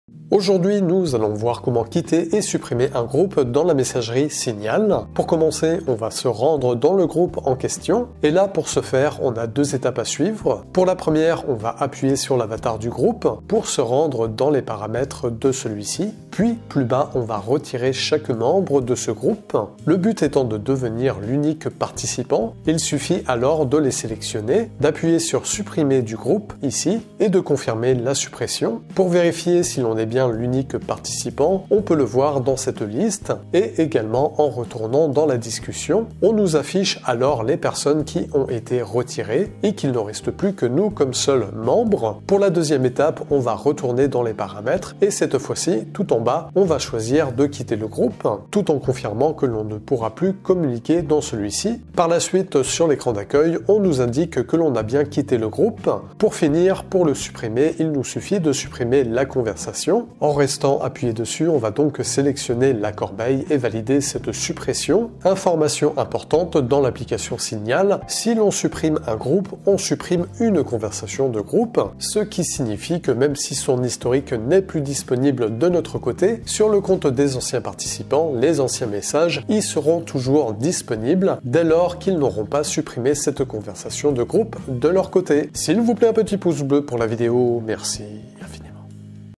SUPPRIMER GROUPE Signal, comment quitter et supprimer un groupe sur Signal Messenger
Supprimer un groupe Signal, comment quitter et supprimer un groupe sur Signal Messenger.
Aujourd’hui, nous allons voir comment quitter et supprimer un groupe dans la messagerie Signal.
Pour commencer, on va se rendre dans le groupe en question.
Et là, pour ce faire, on a deux étapes à suivre.
Pour la première, on va appuyer sur l’avatar du groupe pour se rendre dans les paramètres de celui-ci.
Puis, plus bas, on va retirer chaque membre de ce groupe.
Le but étant de devenir l’unique participant.
Il suffit alors de les sélectionner.
D'appuyer sur “supprimer du groupe”, ici.
Et de confirmer la suppression.
Pour vérifier si l’on est bien l’unique participant, on peut le voir dans cette liste.
Et également, en retournant dans la discussion.
On nous affiche alors les personnes qui ont été retirées.
Et qu’il ne reste plus que nous comme seul membre.
Pour la deuxième étape, on va retourner dans les paramètres.
Et cette fois-ci, tout en bas, on va choisir de “quitter le groupe”.
Tout en confirmant que l’on ne pourra plus communiquer dans celui-ci.
Par la suite, sur l’écran d’accueil, on nous indique que l’on a bien quitté le groupe.
Pour finir, pour le supprimer, il nous suffit de supprimer la conversation.
En restant appuyer dessus, on va donc sélectionner la corbeille et valider cette suppression.
Information importante, dans l'application Signal, si l’on supprime un groupe, on supprime une conversation de groupe.
Ce qui signifie, que même si son historique n’est plus disponible de notre côté, sur le compte des anciens participants, les anciens messages y sont toujours disponibles dès lors qu’ils n’auront pas supprimer cette conversation de groupe de leur côté.